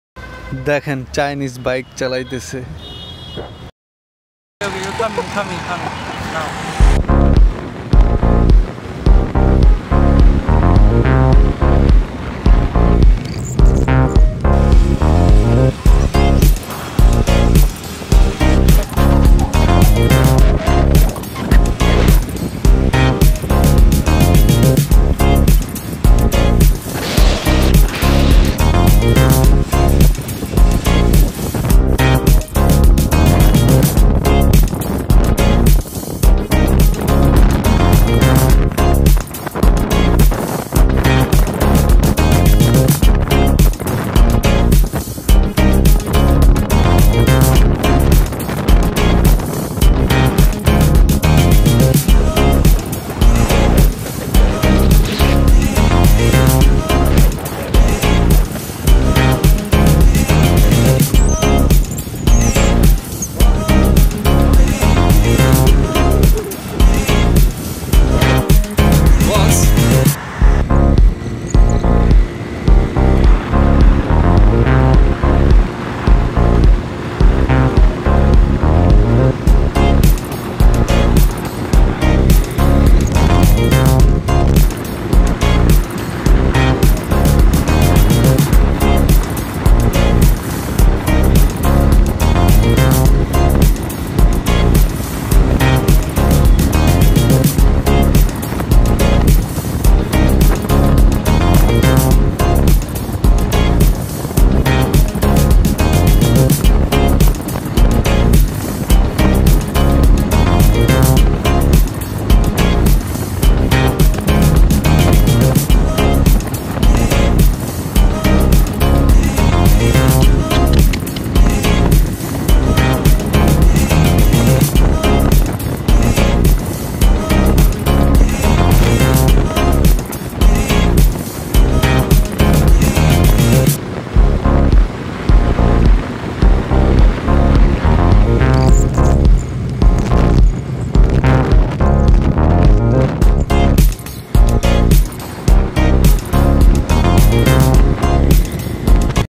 simple blog 02 with chinese people ppp project
simple blog 02 with chinese people ppp project bangladesh
চাইনিজ খুব সুন্দর ড্রাইভ করে হুম চাই খুব সুন্দর ড্রাইভ করে